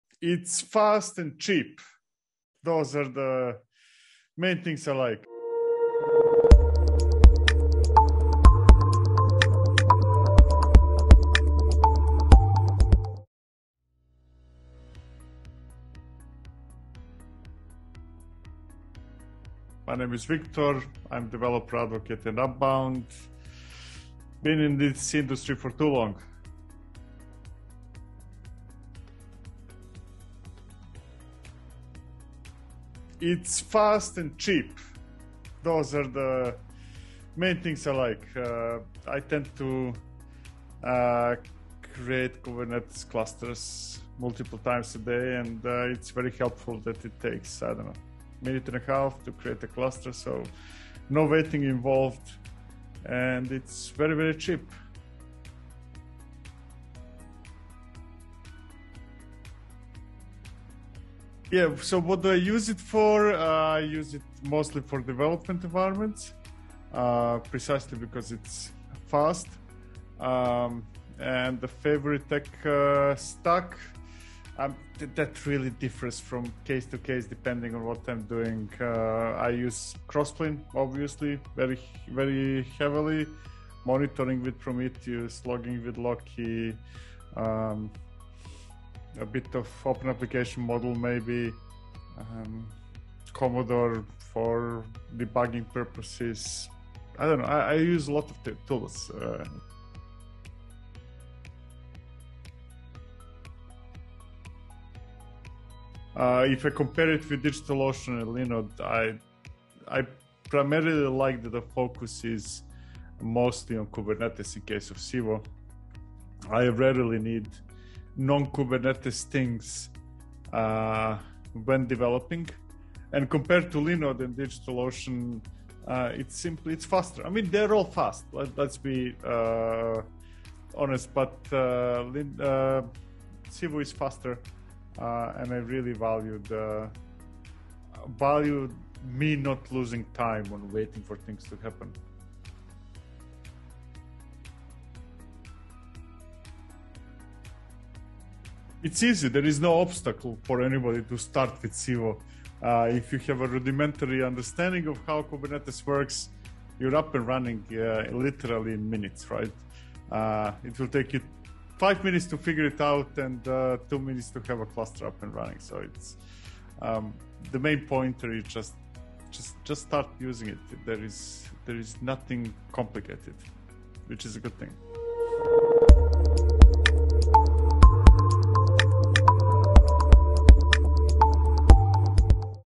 “Civo is faster than DigitalOcean and Linode” - Viktor Farcic from Upbound
“Civo is faster than DigitalOcean and Linode”. Learn what Viktor Farcic from Upbound has to say about Civo and how it helps in their day-to-day development.

Timestamps:
0:00 Teaser
0:05 Introduction
0:27 What attracted you to Civo?
0:55 What do you use your clusters for?
1:39 How does Civo compare to other cloud providers?
2:22 Advice for someone looking to move to Civo?
2:55 Outro